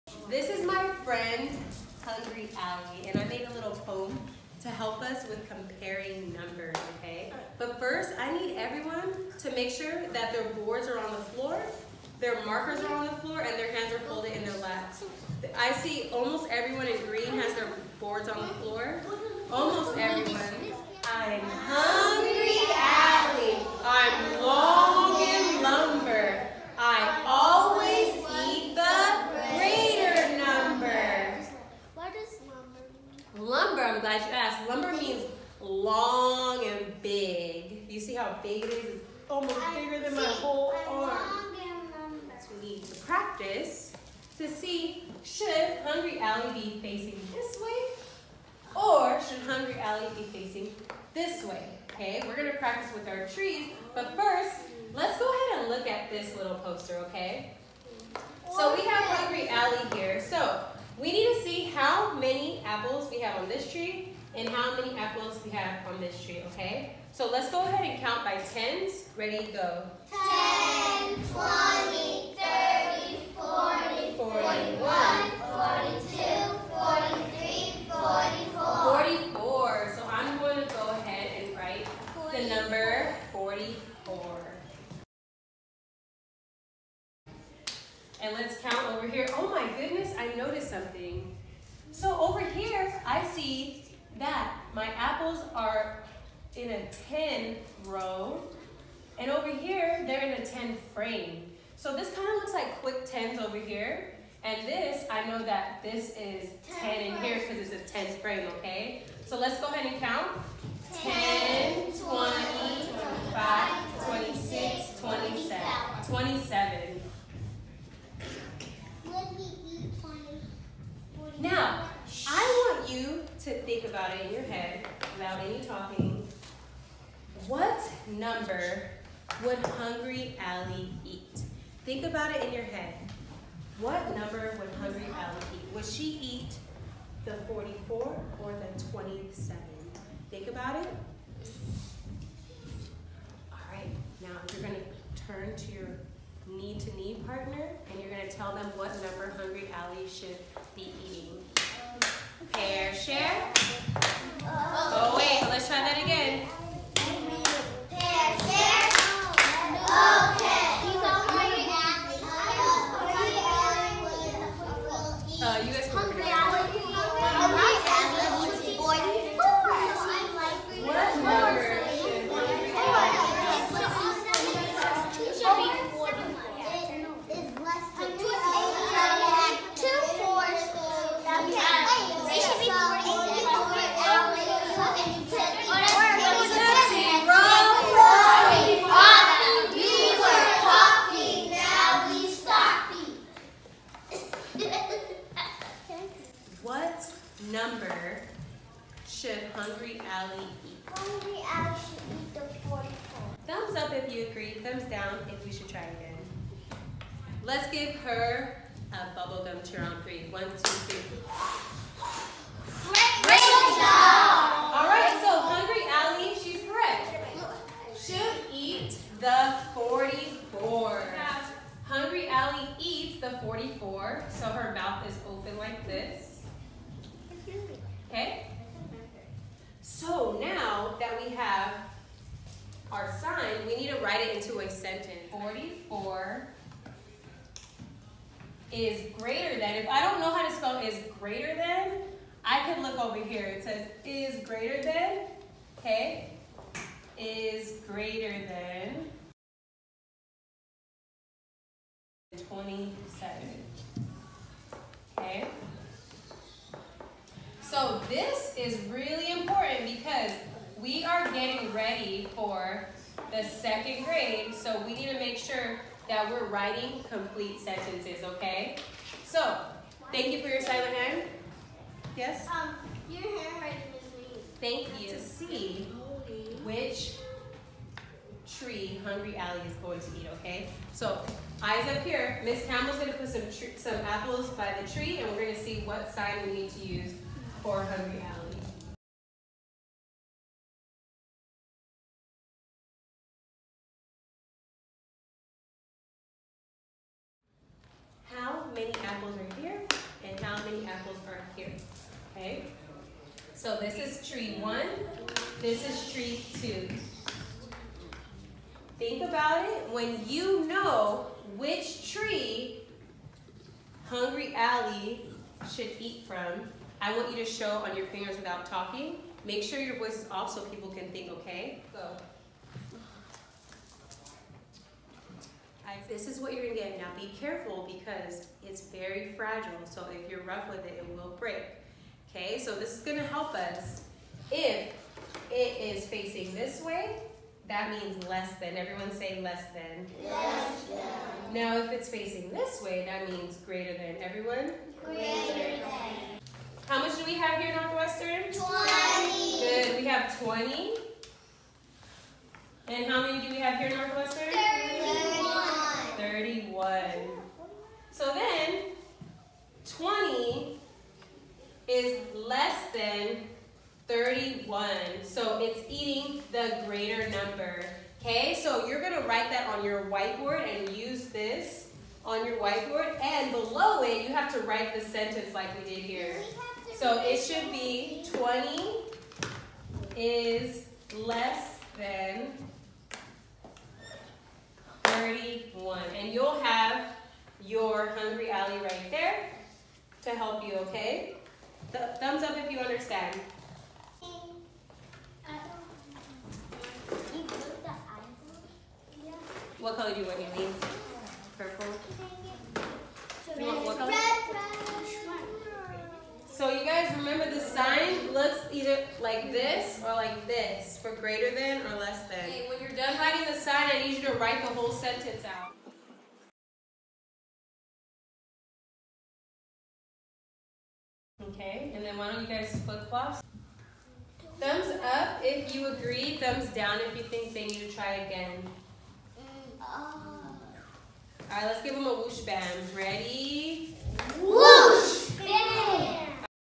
How To Compare Numbers - Greater Than, Less Than. First Grade Math Lesson
Here is a fun poem that will help your scholars remember which way the greater than and less than sign should be facing.

I’m hungry Allie, I’m long and lumber, I always eat the greater number.

Each scholar also got their own Allie made out of popsicles sticks, and googly eyes to help with this concept.